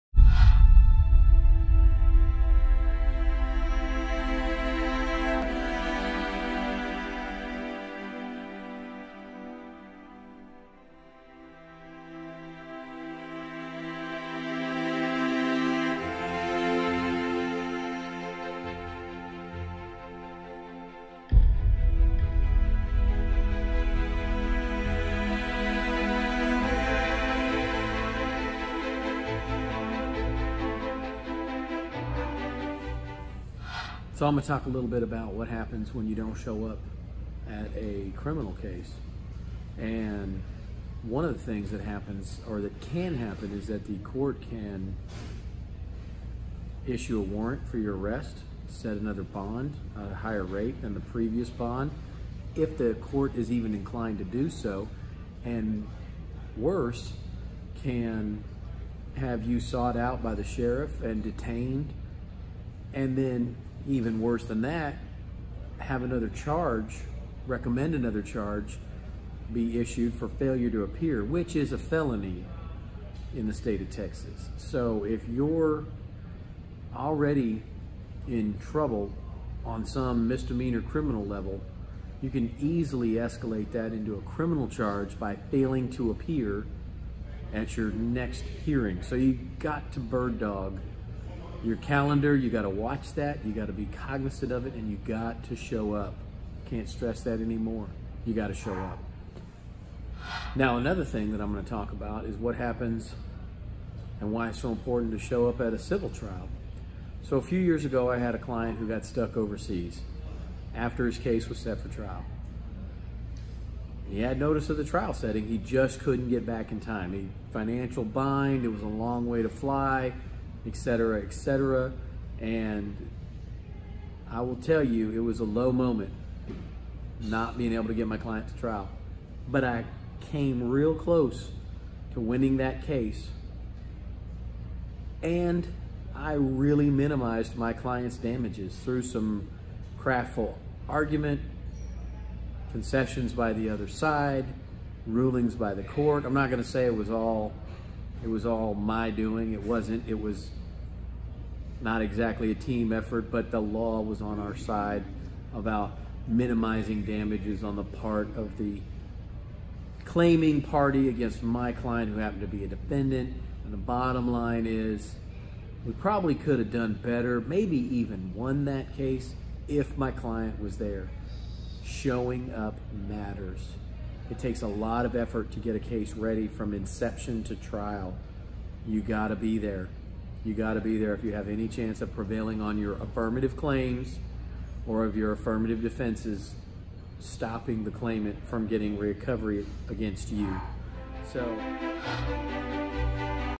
Next in our short video series on how important it is to show up to court!
Next in our short video series on how important it is to show up to court! Showing up is 90% of life. Actually, it could be more like 100% of life, depending on who you ask. This video series explores what can happen when you don't show up to court in criminal and civil cases.